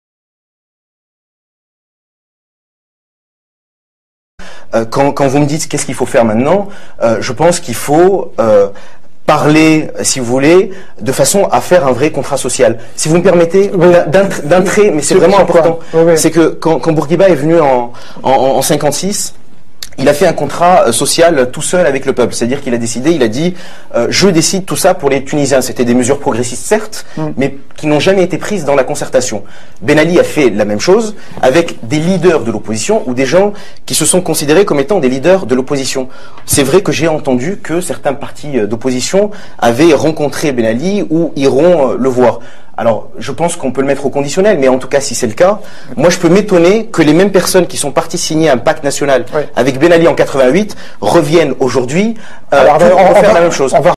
p3-9 Selim Ben Hassen Président de Byrsa Arrêt sur images 14 janvier 2011
Extrait de Selim Ben Hassen. Selim Ben Hassen : portrait d’un tunisien engagé.Pour peu qu’on s’intéresse à la vie étudiante à Sciences Po, on a forcément eu l’occasion de discuter un jour avec Selim Ben Hassen. Celui qui a fondé Sciences Po Monde Arabe fait en effet partie de ces quelques étudiants qui, s’étant beaucoup impliqués dans la vie associative, bénéficient des égards de la Direction et d’une certaine notoriété auprès des élèves. Mais le parcours de Selim Ben Hassen est aussi celui d’un étudiant déterminé à mener un engagement politique pour son pays